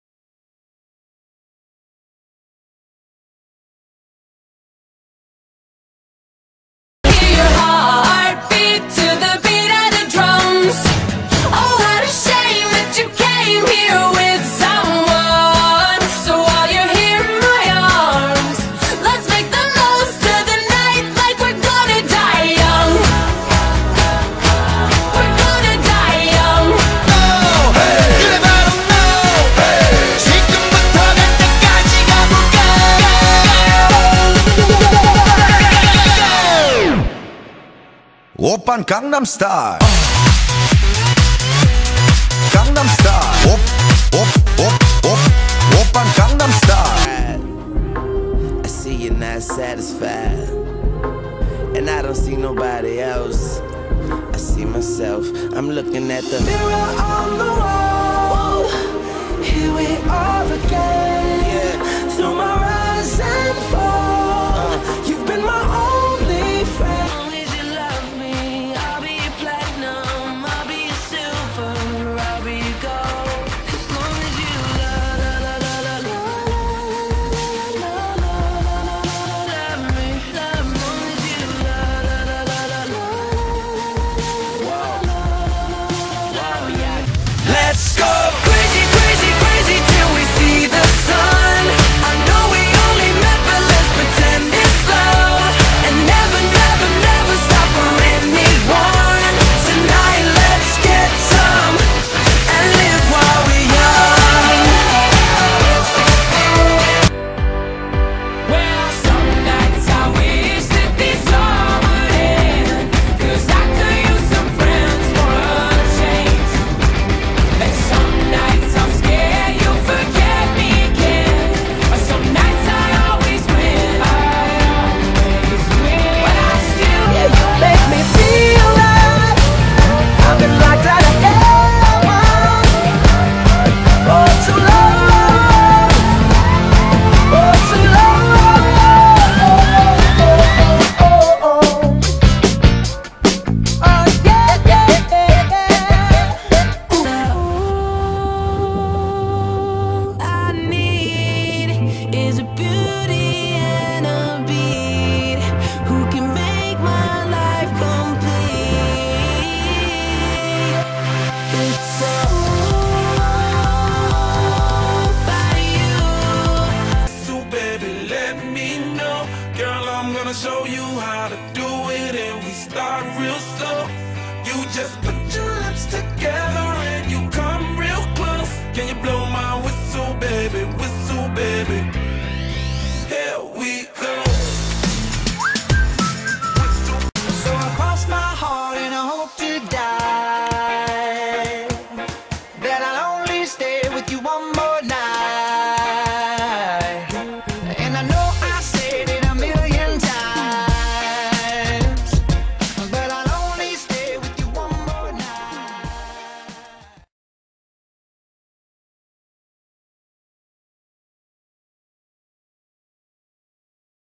Top 10 Songs: December 23rd, 2012 (Episode 3)
Hope you enjoy episode 2 of Top 10 Songs! Hope you like these songs! Be sure to tell me good songs for me to listen to in the comments and what you think of these songs! If I like the songs you you tell me to listen to I might put them in the next episode.
P.S: Yes, I know that for #3 I spelt Bieber wrong. I spelt beiber.